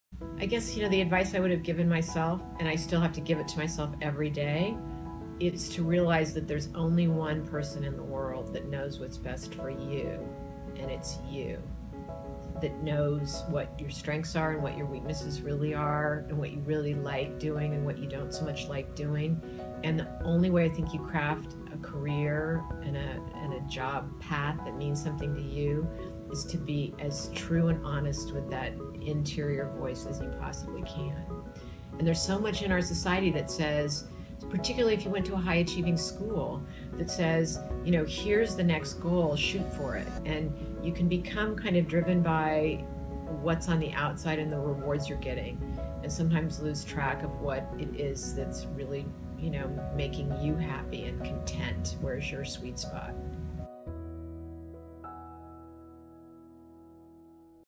One person knows what's best for you (and it's you) | Kate Grant *94
Kate Grant *94, is the CEO of the Fistula Foundation, a non-profit that treats obstetric fistula for the poorest women in the world.

Q. What advice would you give to that 21-year-old?

A. I guess the advice that I would have given myself, and I still have to give it to myself every day, is to realize that there's only one person in the world that knows what's best for you, and it's you. That knows what your strengths are and what your weaknesses really are, what you really like doing and what you don't so much like doing. The only way to craft a career and job path that means something to you is to be as true and honest with that interior voice as you possibly can. There's so much in our society that says, particularly if you went to a high-achieving school, that says, ‘Here's the next goal shoot for it,’ and you can become kind of driven by what's on the outside and the rewards you're getting, and sometimes lose track of what it is that’s really making you happy and content.  Where’s your sweet spot?

CAREER & LIFE VISION CONVERSATIONS is a video series by Princeton Career Services highlighting lessons learned by inspiring Princetonians.
Produced by Lisa Einstein '13